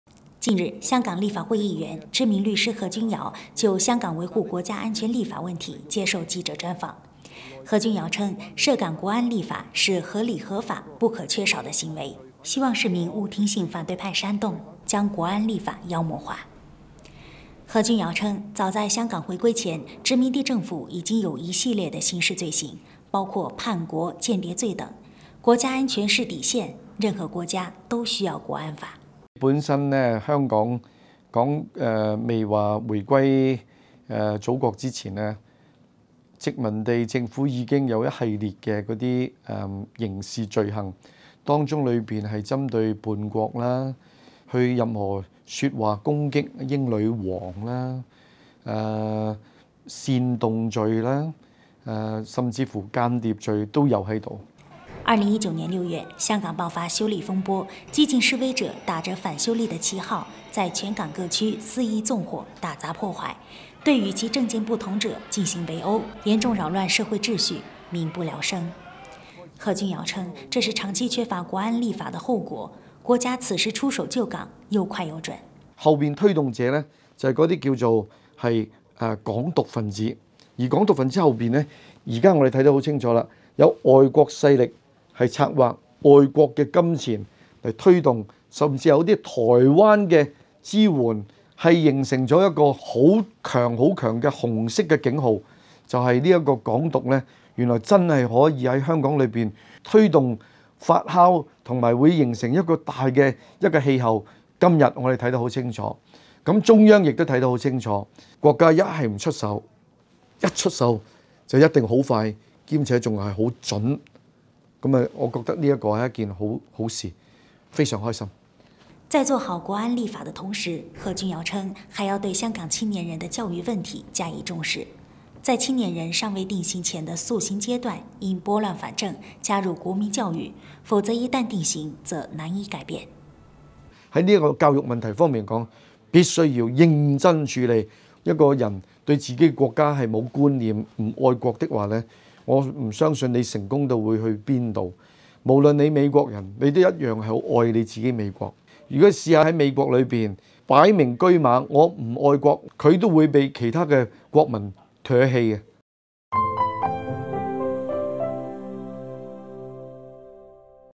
何君尧：香港回归前殖民地政府已有针对“叛国”刑事罪行
近日，香港立法会议员、知名律师何君尧就“香港维护国家安全立法”问题接受记者专访。何君尧称，“涉港国安立法”是合理合法、不可缺少的行为，希望市民勿听信反对派煽动，将“国安立法”妖魔化。何君尧称，早在香港回归前，殖民地政府已经有一系列的刑事罪行，包括“叛国”、“间谍罪”等。“国家安全”是底线，任何国家都需要“国安法”。